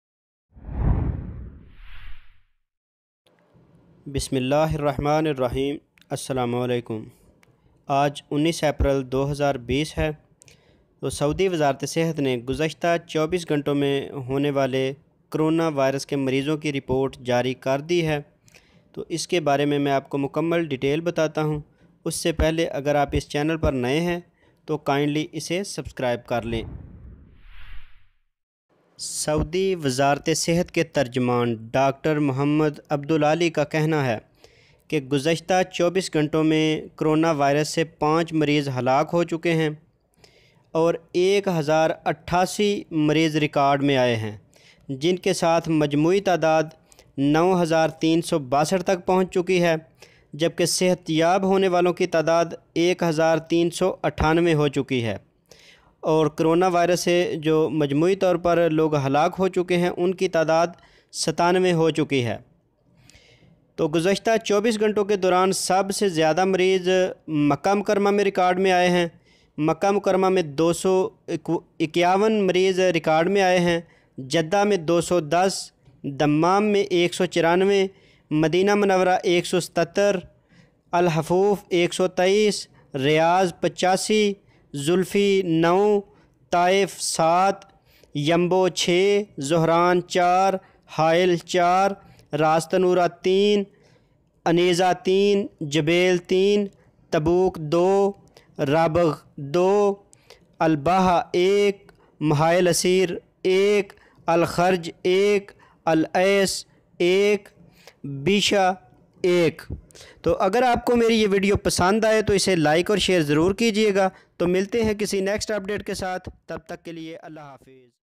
Ministry of Health latest news in Urdu | Ksa info tv (19/04/2020)
Is video mein main aapko Corona virus ke hawale se Saudi Arab mein nai marizon ki tafseel bataunga